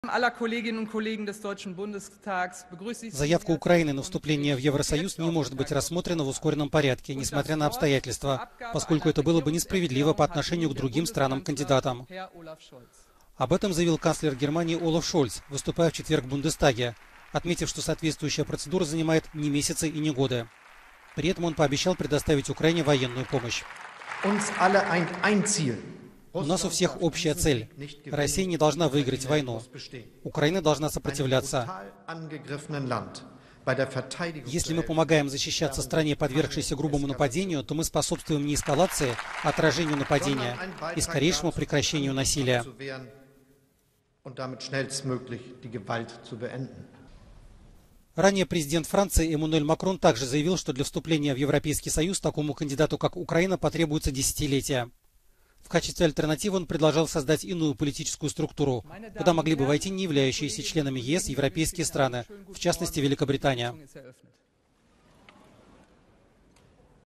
Вступление в ЕС: "вопрос десятилетий"
Для принятия Украины в Европейский союз потребуются «не месяцы и не годы». Об этом заявил канцлер Германии Олаф Шольц.